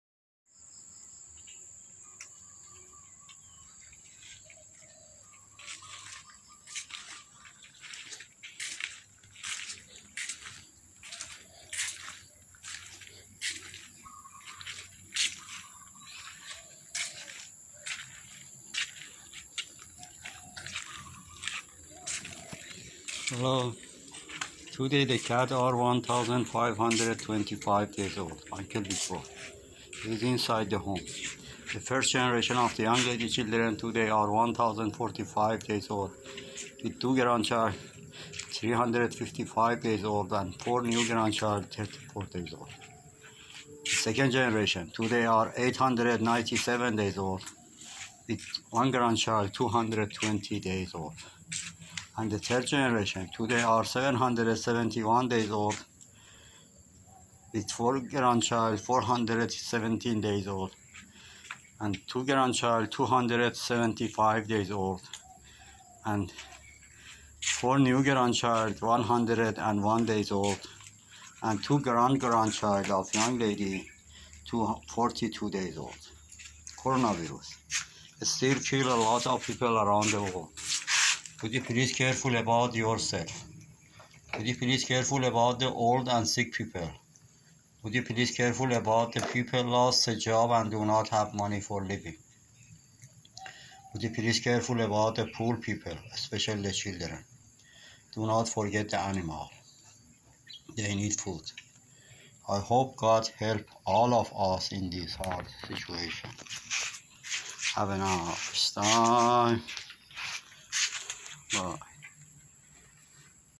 Time Machine Cats !!!! Longest Daily Cat's Story
The story of this Channel is about a female cats , her name is Young Lady.
Today cats are1525 days old , and 3 Generation of Young Lady children are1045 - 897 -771 and Young lady grandchild are 355 , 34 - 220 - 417 , 275 , 101 and grand grandchild are 42 days old .